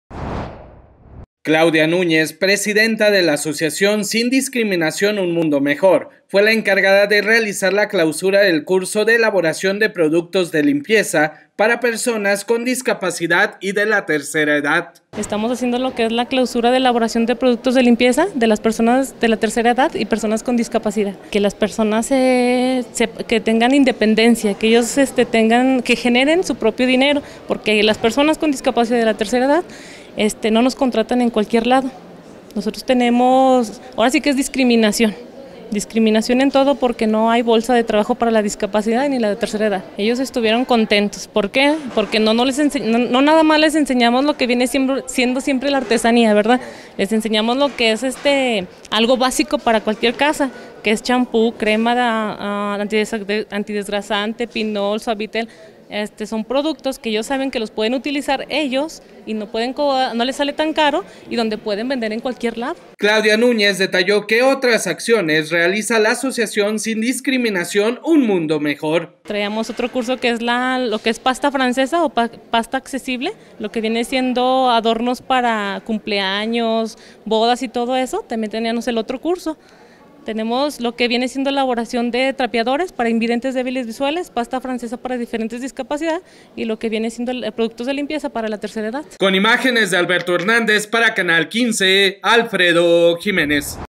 Participan 20 personas en el curso de elaboración de productos de limpieza
Claudia Núñez, presidenta de la Asociación Sin Discriminación Un Mundo Mejor, fue la encargada de realizar la clausura del Curso de Elaboración de Productos de Limpieza para Personas Con Discapacidad y de la Tercera Edad.

Claudia Núñez, detalló que otras acciones realiza la Asociación Sin Discriminación Un Mundo Mejor